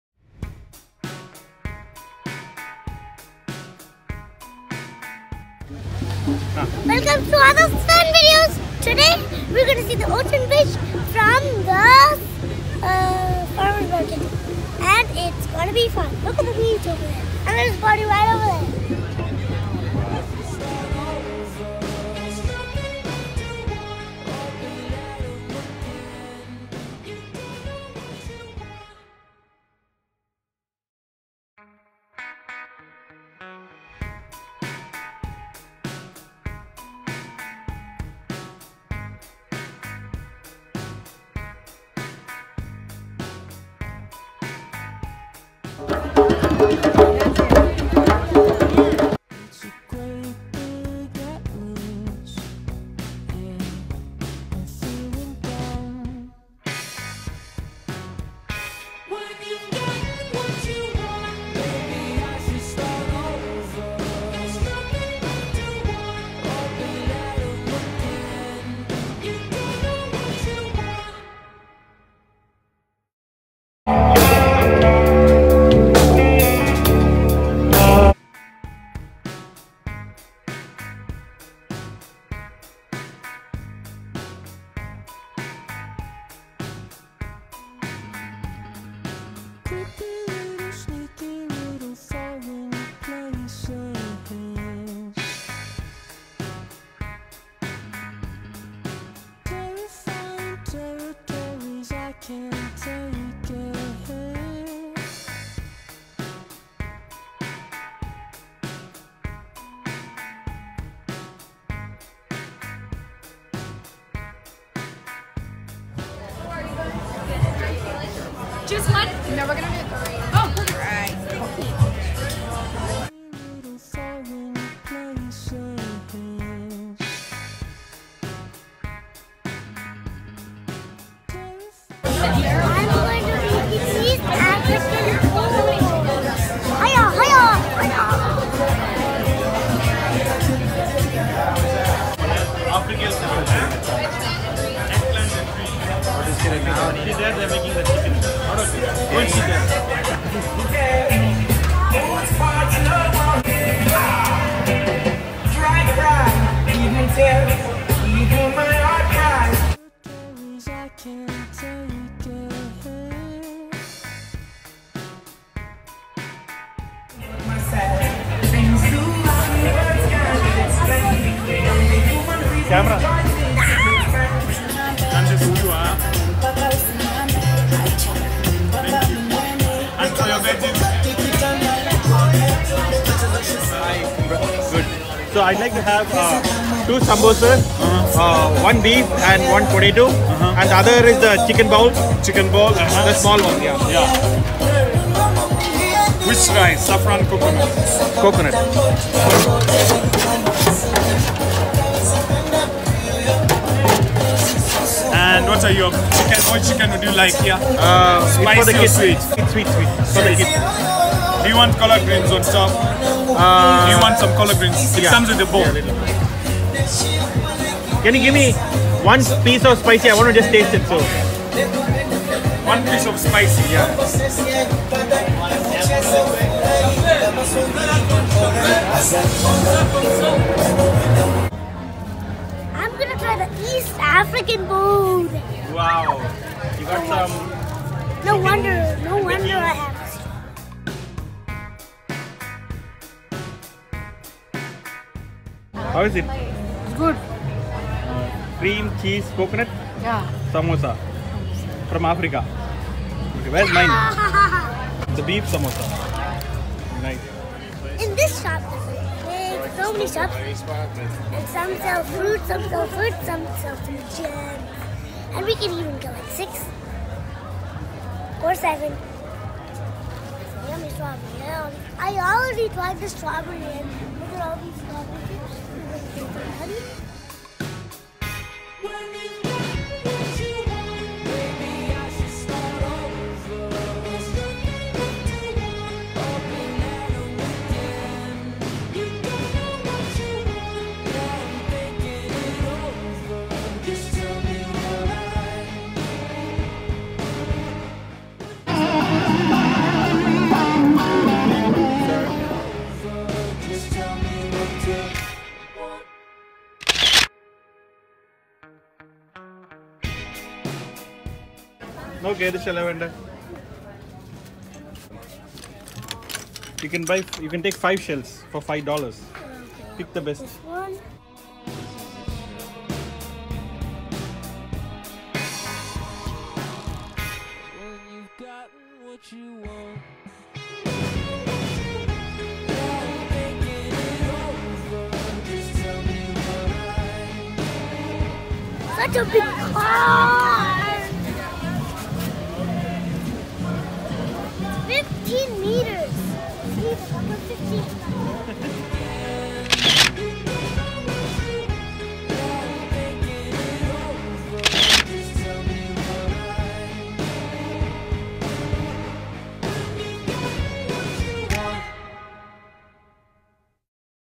Adhav’s Fun Videos|| Ocean Beach Farmers Market || San Diego
Loved the beach, the farmers market - had yummy east African food, bought the freshest strawberries and also got to buy some starfishes and sea shells